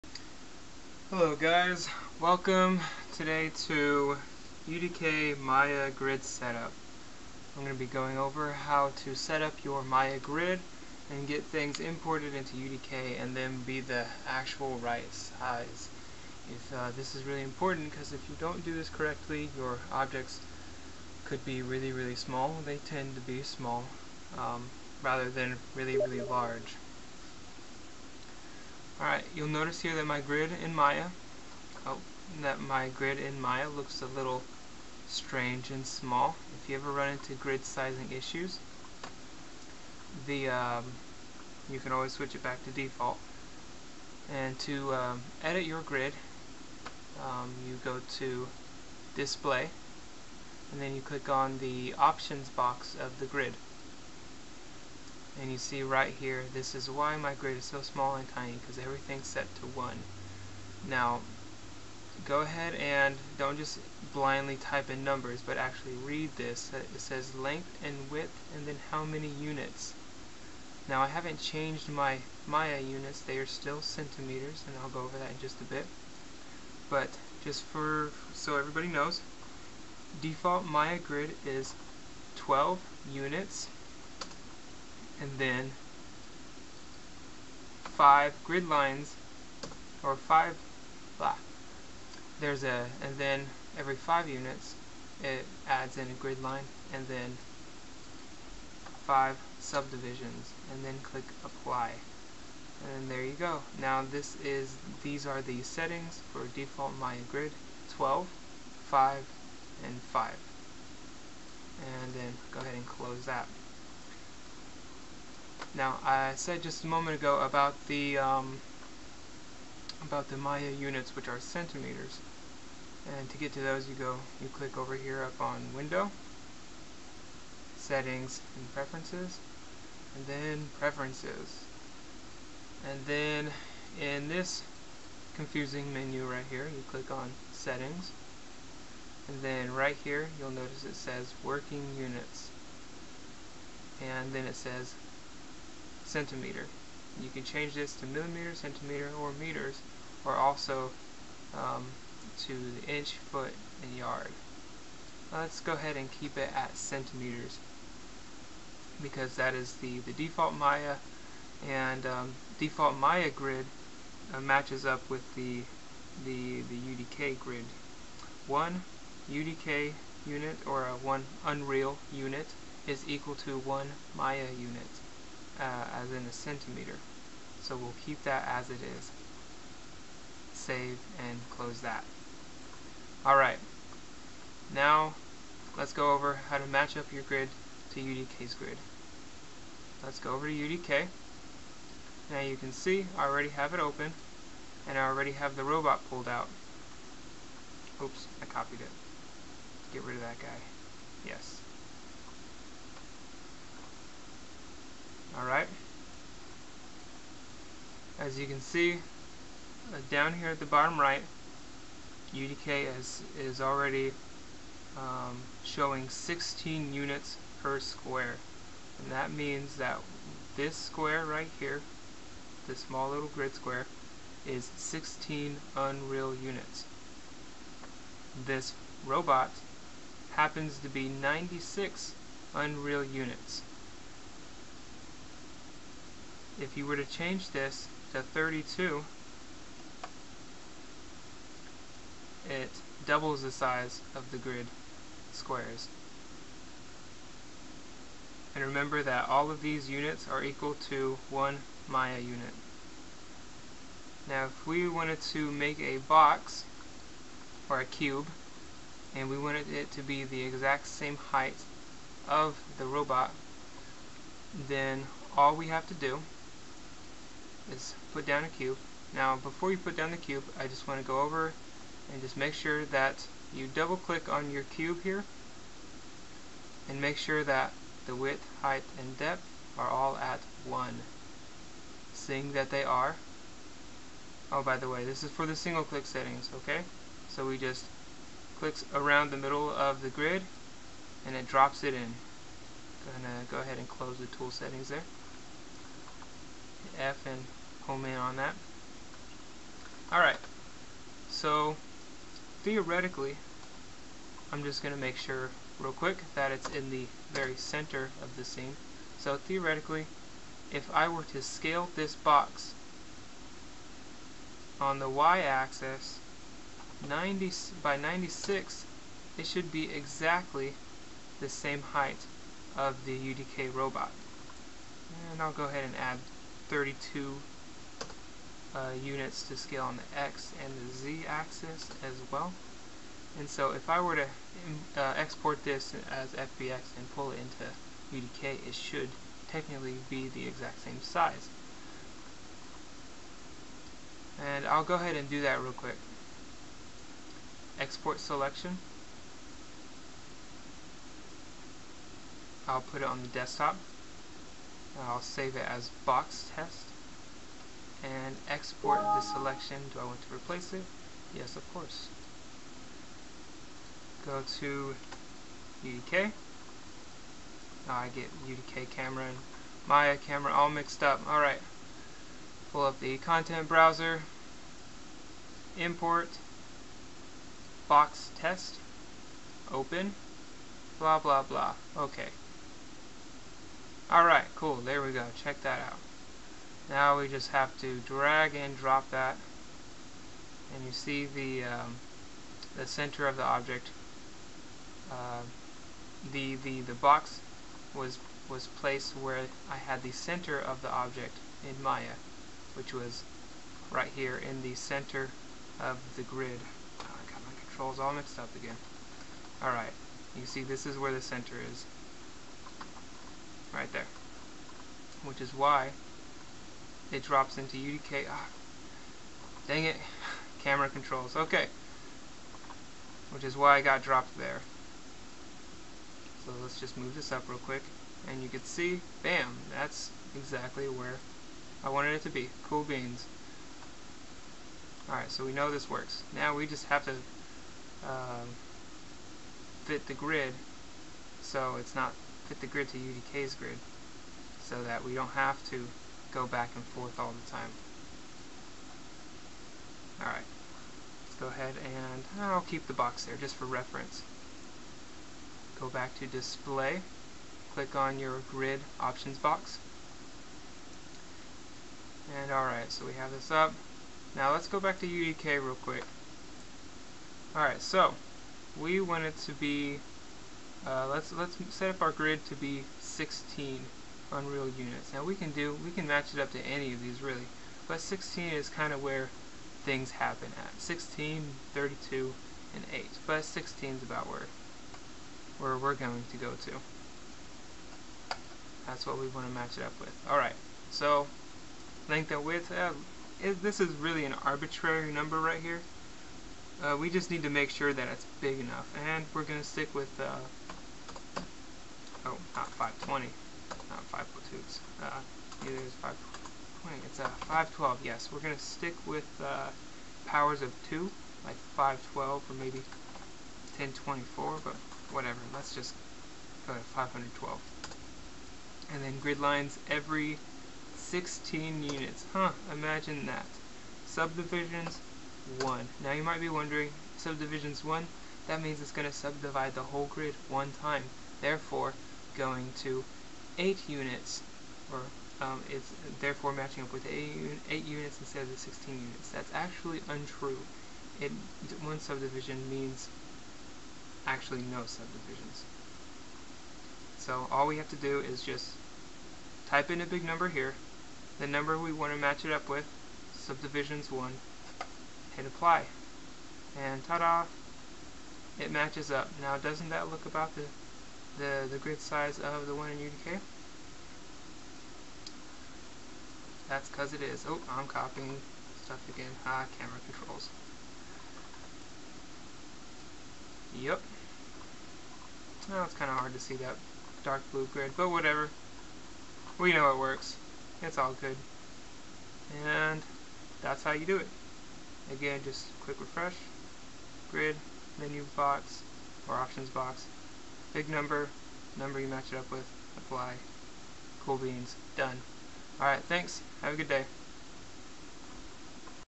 Comprehensive - How To Setup Maya Grid for UDK
I forgot to mention, for Ruler Tool to check your size, click Create---Measure Tools---Distance Tool. To use, Left Click to set the first point, then again for the second point. You can snap these points to the grid as well.